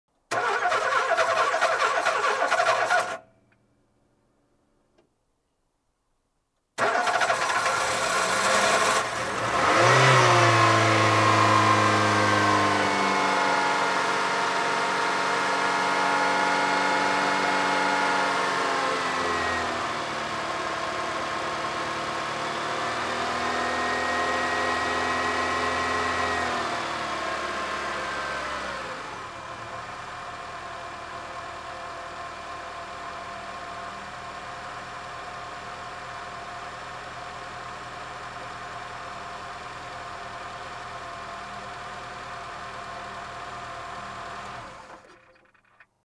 bruit de moteur de voiture, demarrage difficile
Démarrage difficile d'une voiture à essence sans plomb 95. Il s'agit d'une Peugeot 106.
credit sardin
bruit de moteur de voiture,bruitage moteur voiture,bruitage demarrage voiture,moteur de,moteur de voiture,moteur de voiture comment ca marche